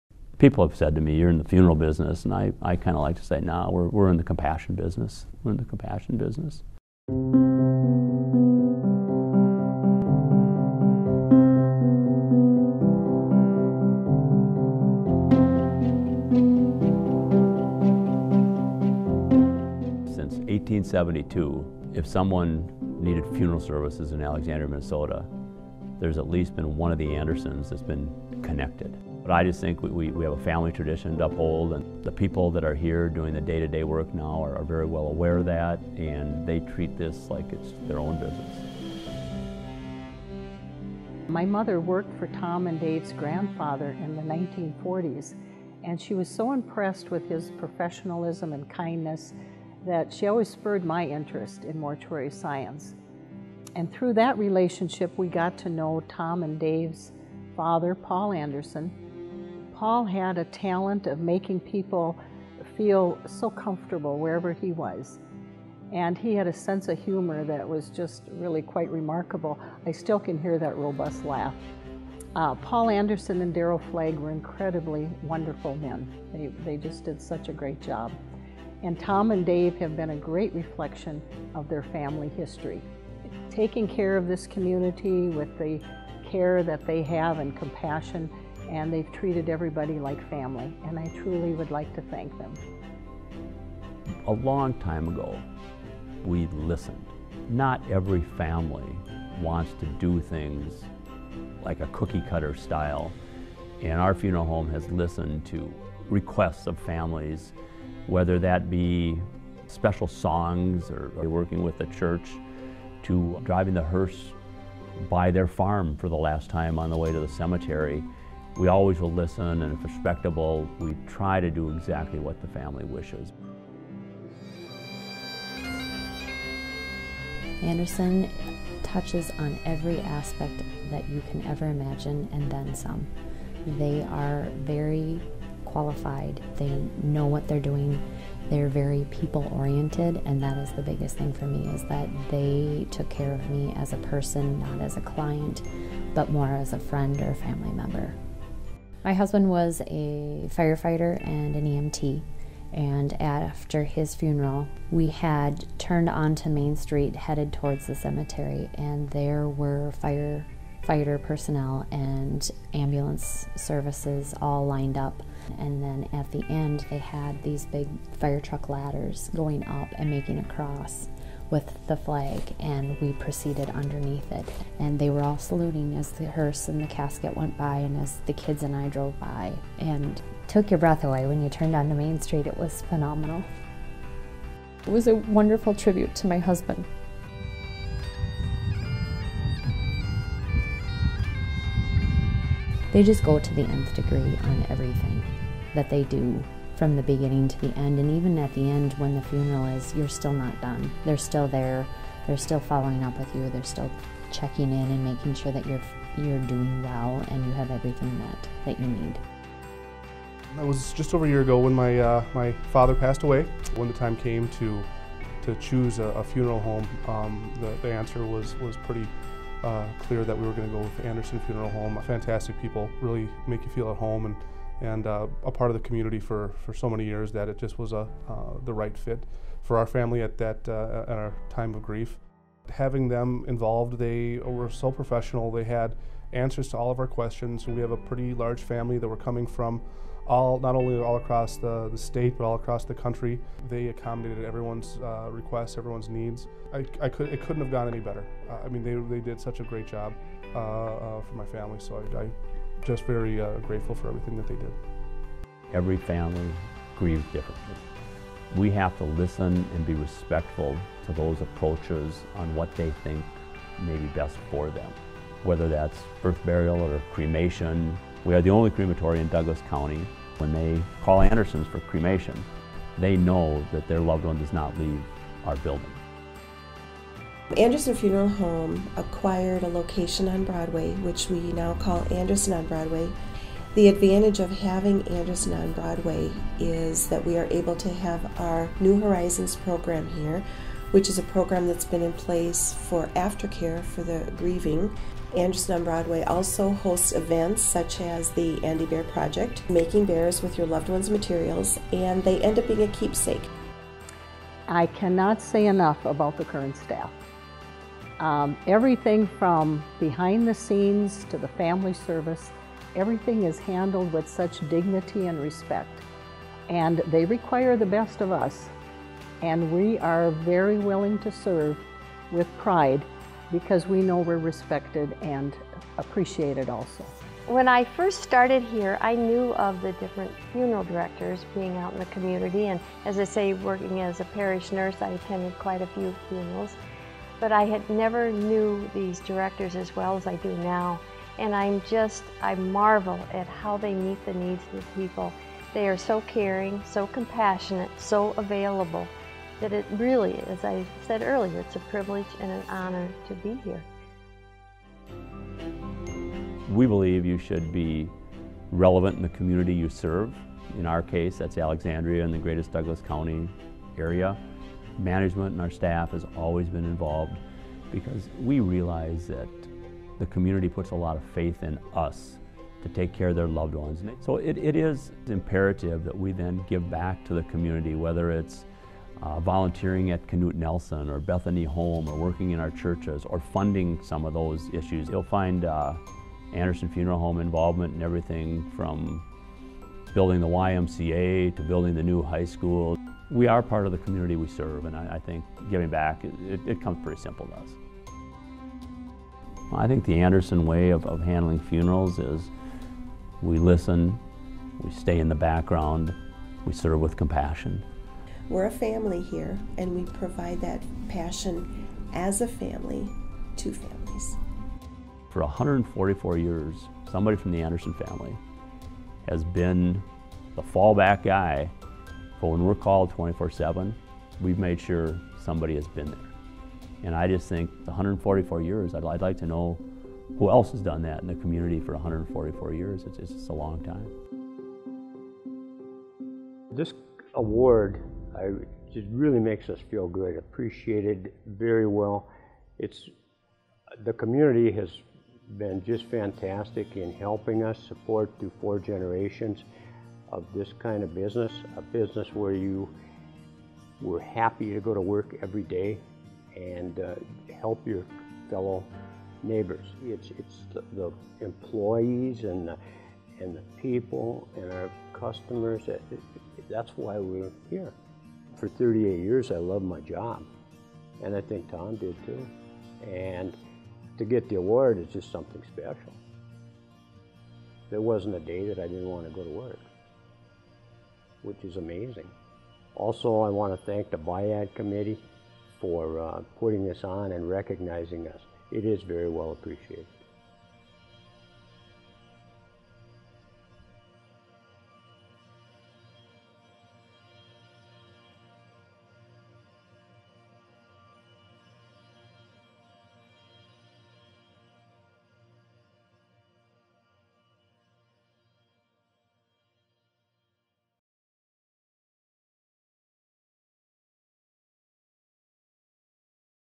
Anderson Funeral Home & Crematory - Business & Industrial Appreciation Day 2017
livingalexarea.org - Anderson Funeral Home is the recipient of the 2017 Business & Industrial Appreciation Day award by the Boards of Directors of the Alexandria Area Economic Development Commission and Alexandria Lakes Area Chamber of Commerce.

The Business & Industrial Appreciation Day (BIAD) event has honored a Douglas County company each year since 1984, and is recognized as one of the largest and longest standing annual events of its type in greater Minnesota.  Annually, one company from the region is selected as the BIAD recipient based upon the following factors:

• Growth in number of employees; the impact of the business on the job market
• Increase in sales and/or unit volume; an indication of continued growth
• Capital Investment; an indication of commitment by the business to increase community capacity
• Community Contributions; evidence of the business’s investment of time, skills, and resources to assist in community oriented projects